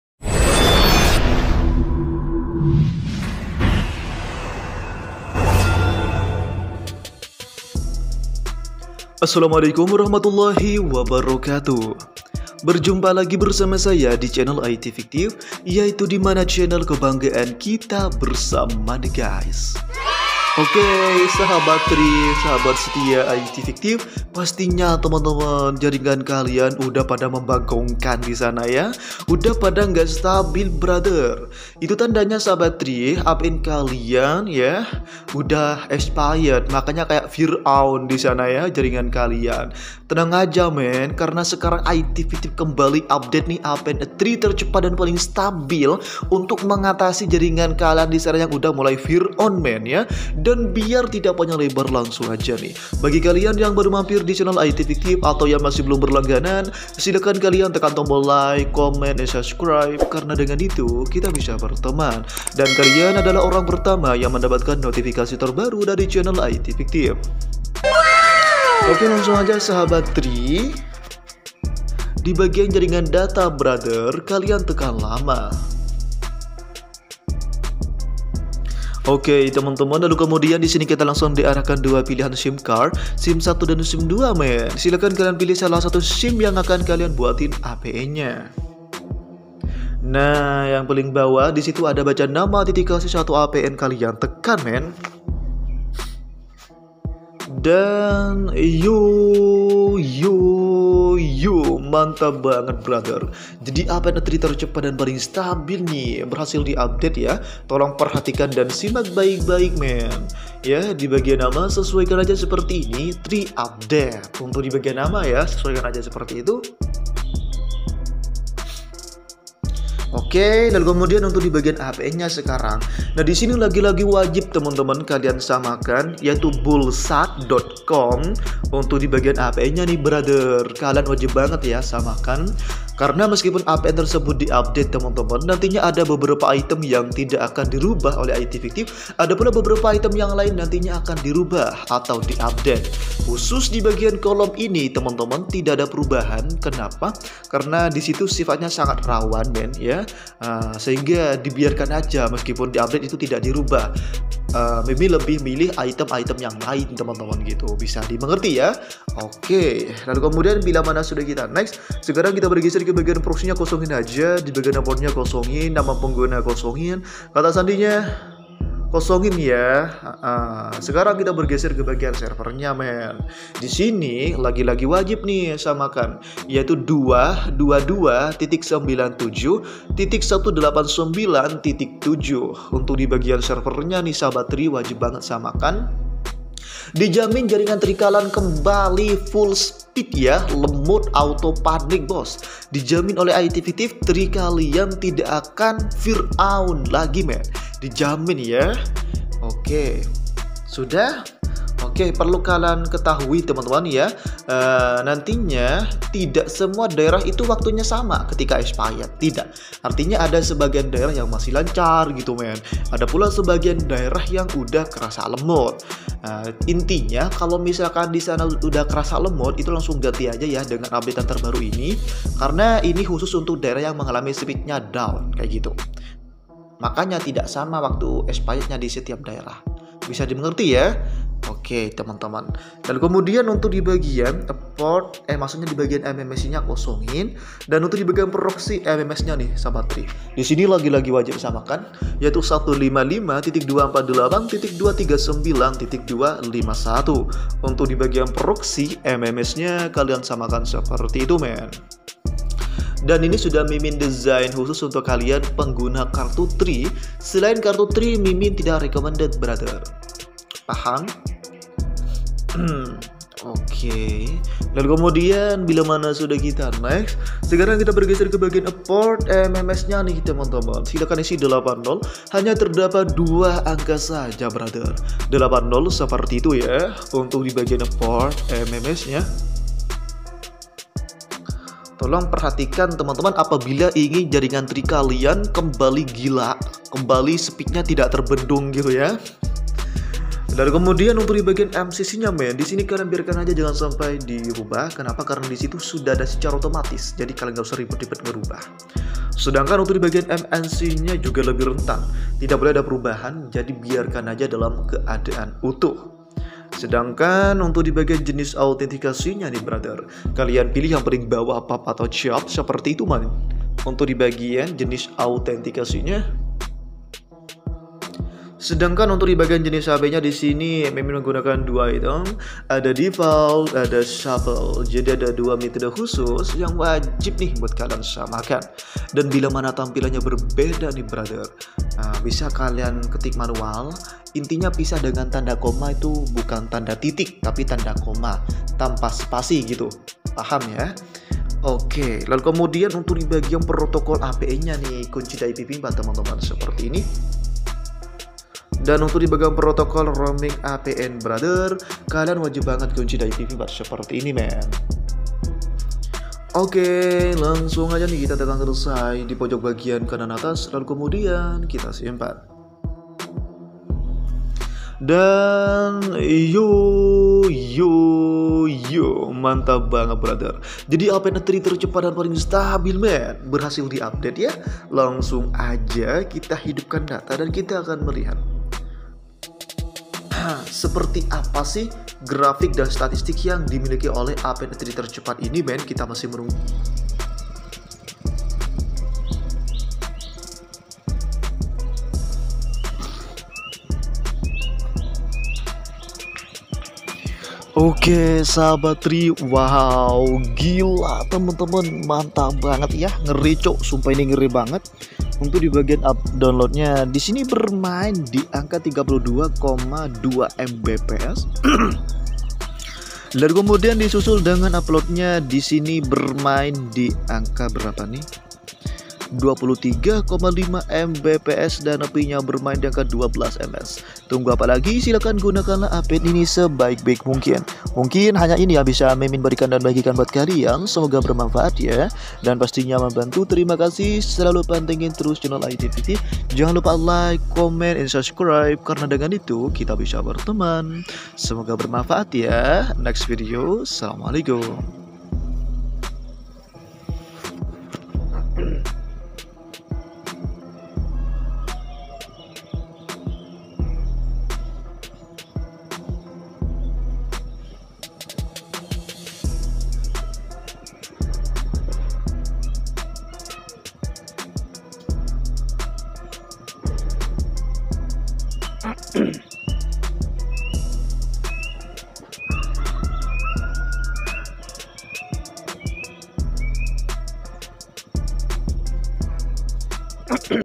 Apn Tri Tercepat Paling Stabil Lemot Kocar-Kacir Speednya Berani Di Adu
🔥 Vedio ini Adalah Cara Seting acces point Name Untuk Memperkuat Sinyal untuk Tri Yang Lemot Setiap TKP Beda Rasa Jngan Takut Untuk Mencoba Semangat sahabat IT FIKTIF